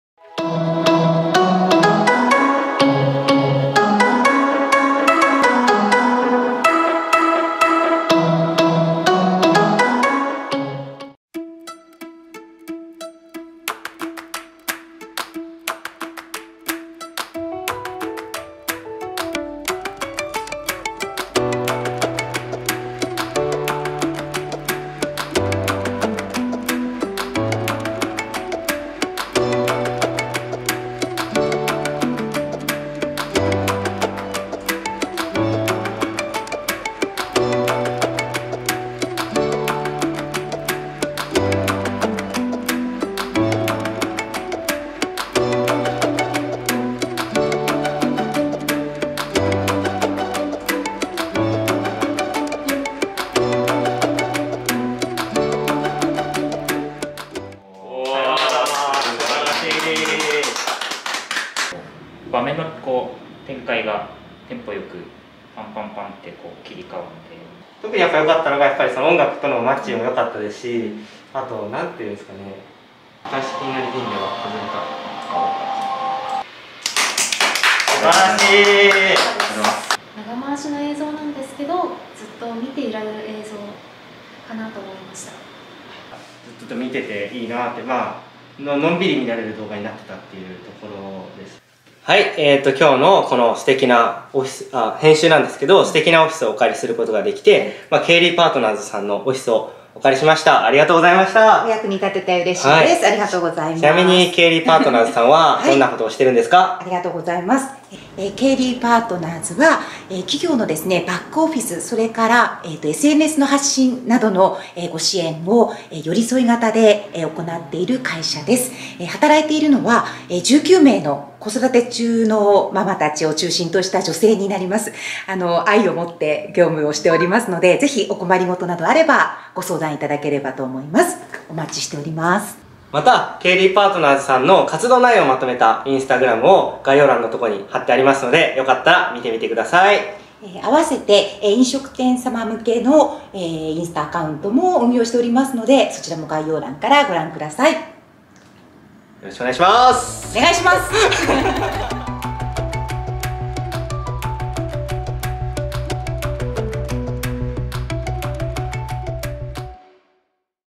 福島VRプロジェクト認定！VRGrapher養成講座
11月29日に行われたVRGrapher養成講座『撮影の部』の続きになる『編集の部』をケイリーパートナーズ様のオフィスをお借りして実施しました！

当日は、福島・・・日本で最初のVRGrapherである当プロジェクト代表の片岡より、VR動画の編集方法についてレクチャーさせて頂きました。

新たに加わったVRGrapherの作品は近日公開予定！

会場を提供頂いたケイリーパートナーズ様のInstagramアカウント